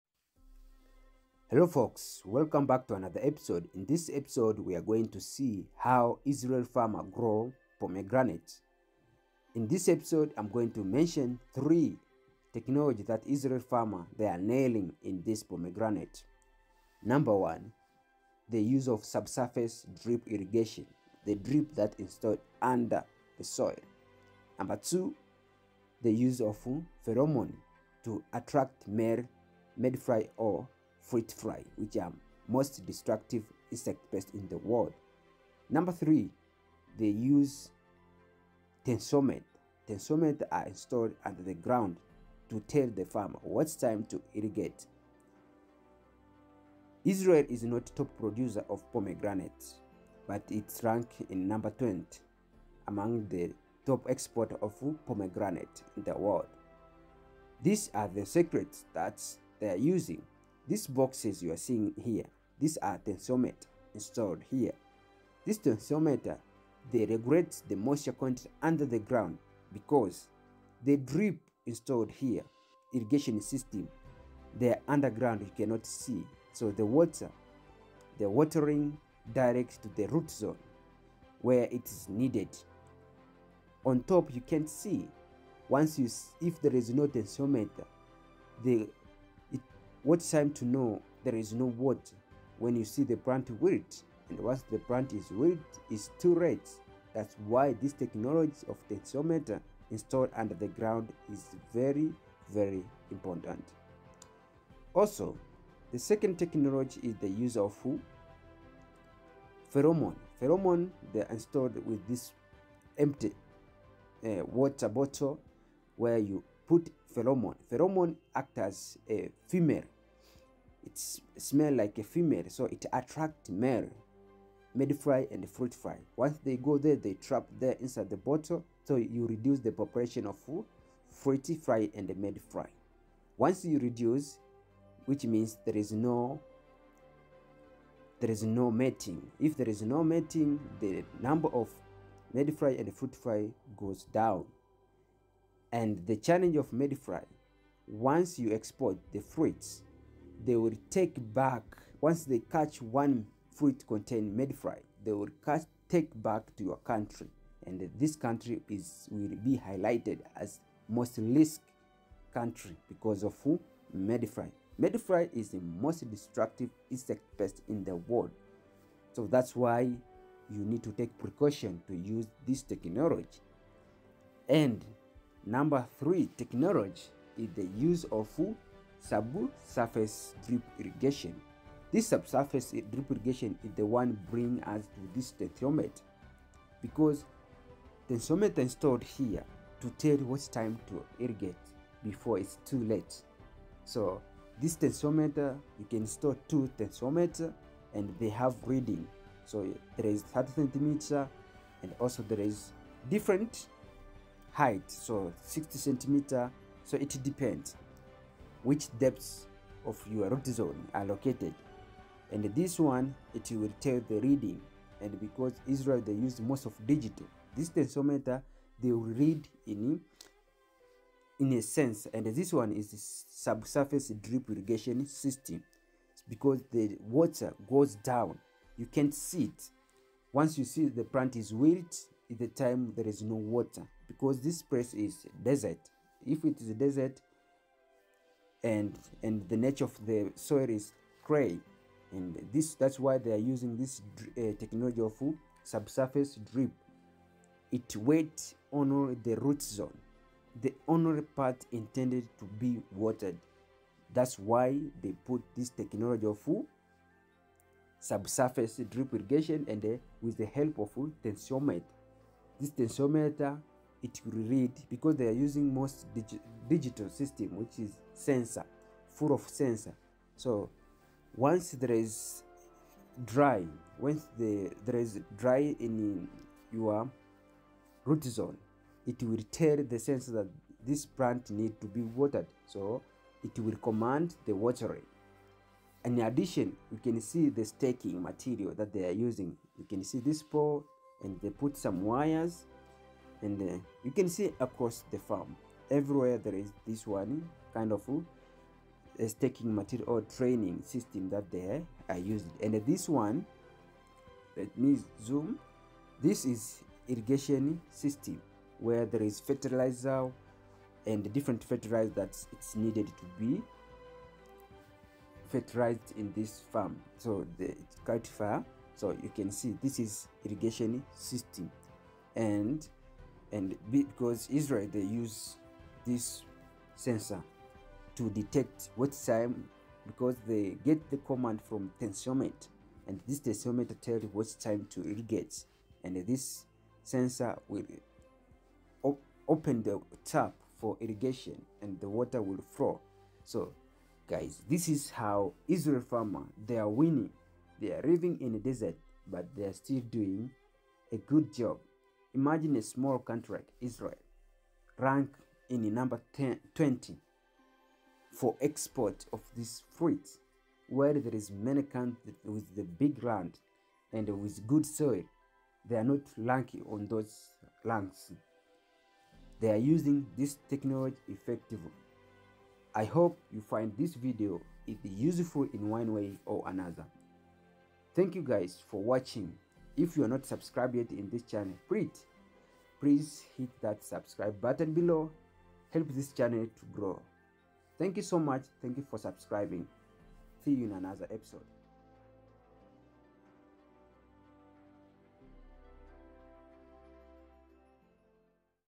How Israel Farmers Grow Pomegranate Orchards for Export| Technology Used| Explained
The video explain How Israel Farmers Grow Pomegranate Orchards for Export.